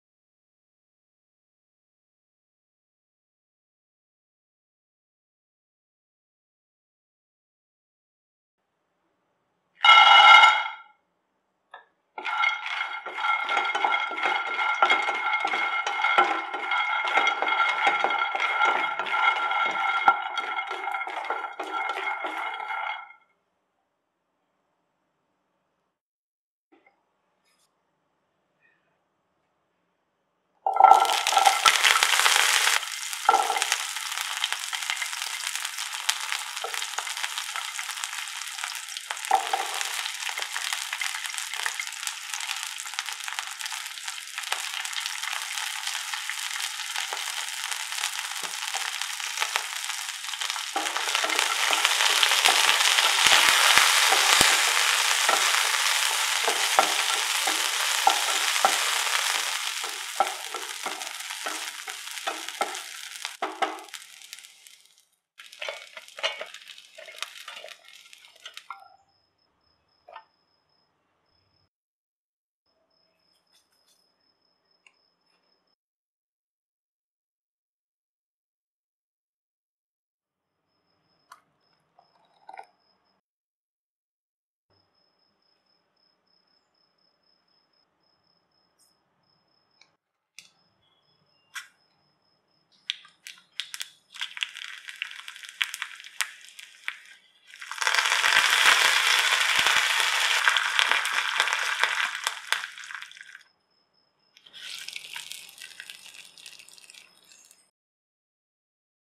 Peanut Chutney Recipe/ Easy Chutney For Idli, Dosa/ Verkadalai Chutney
Peanut Chutney

Ingredients
Raw peanut – ½ cup
Curry leaves
Coriander leaves
Tamarind
Chopped coconut – 3 tbl spoon
Salt
Mustard – ½ teaspoon
Urad dal – ¾ teaspoon